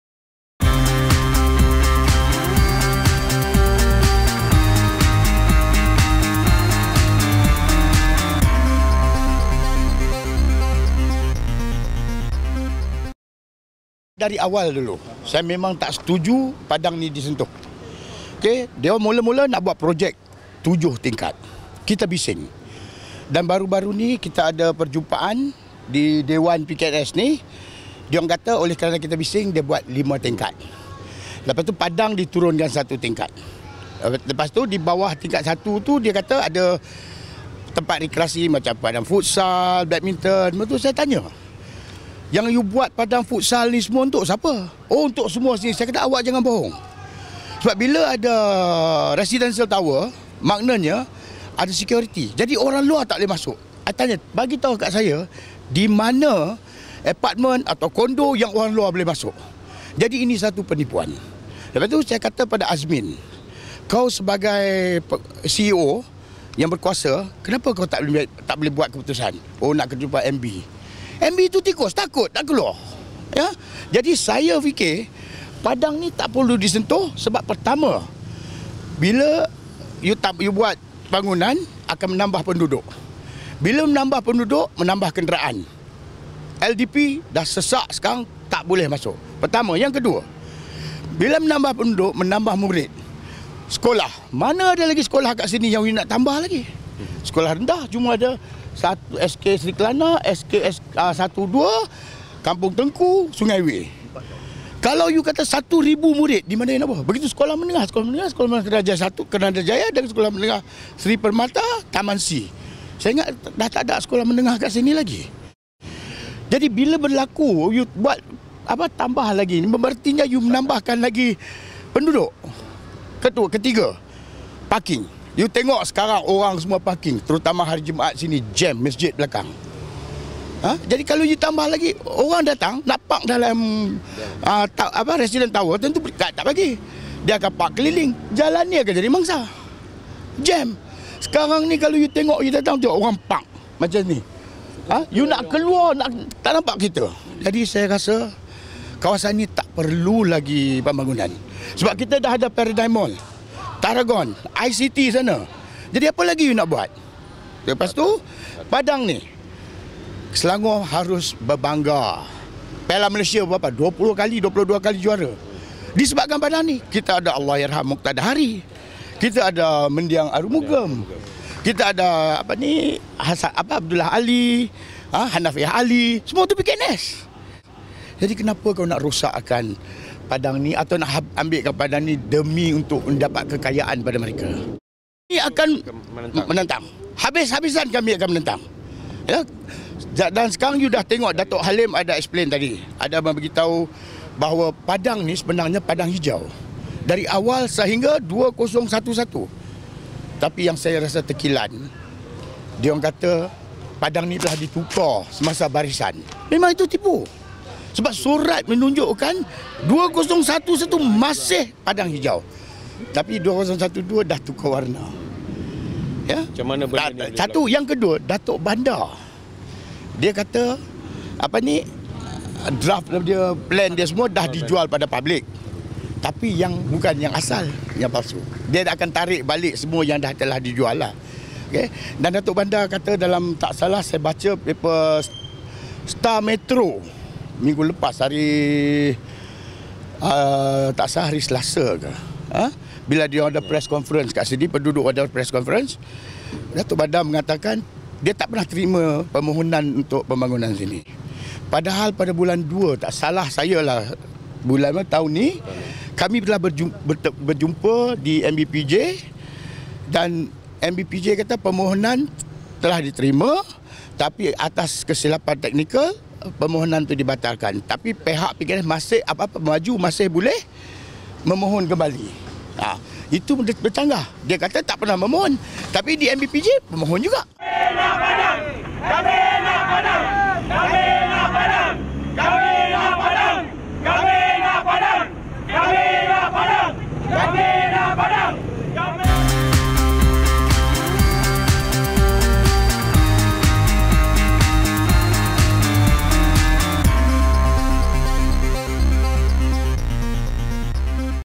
Penduduk Berkeras Menentang Jika Projek Ini Diteruskan - Mohd Aminuddin 3/6/2012
Penduduk Kelana Jaya,En. Mohd Aminuddin Berkata Penduduk akan Berkeras Menentang Jika Projek Diteruskan.Beliau Berkata Demikian Selepas Demo Aman 'Selamatkan Padang PKNS' Di Padang PKNS Seksyen 7,Kelana Jaya.